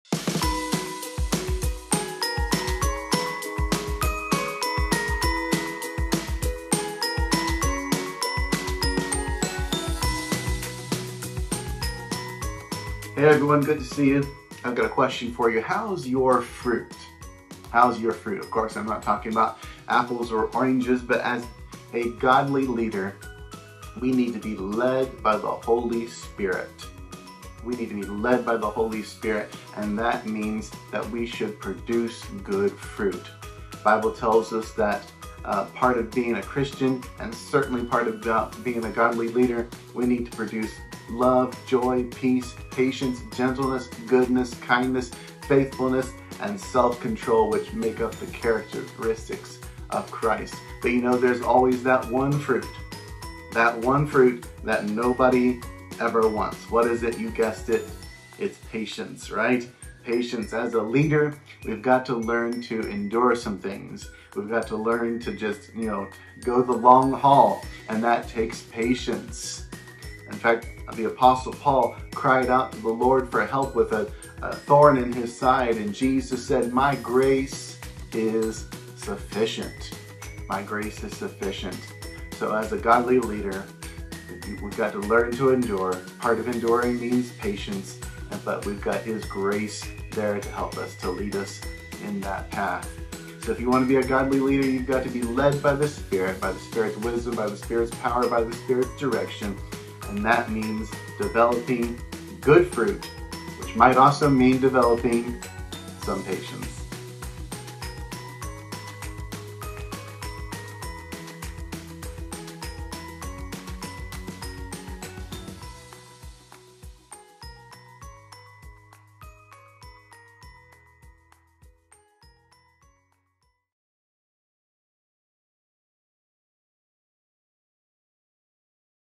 The Fruit You Need But Never Want
How’s Your Leadership Fruit? Godly leadership flows from the wisdom and power of the Holy Spirit.
If you are led by the Holy Spirit you will produce fruit.
There is 1 fruit you need but you never want.
Love, joy, peace, patience, gentleness, goodness, kindness, faithfulness, and self-control make up the characteristics of Christ...